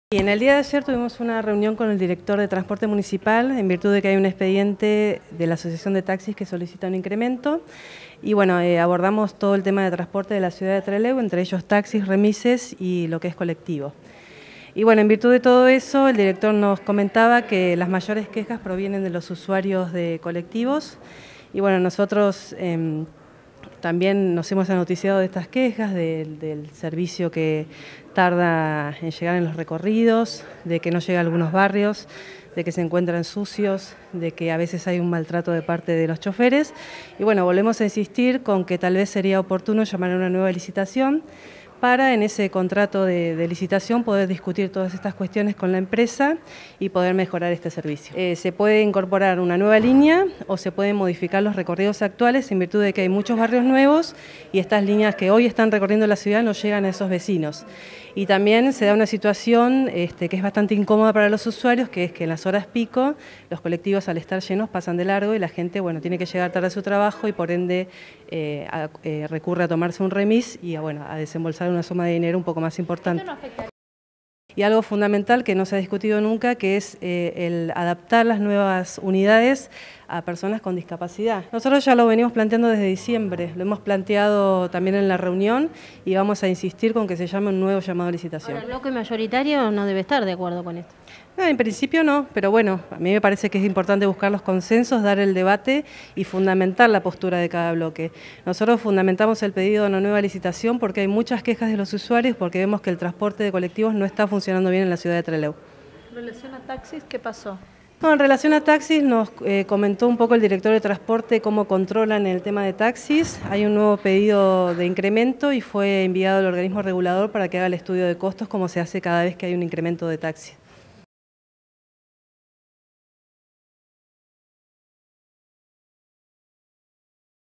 FPV quiere llamar a licitación por transporte de colectivos
Los Concejales de Trelew se reunieron con el Director de Transporte trataron tema taxis remis y colectivos.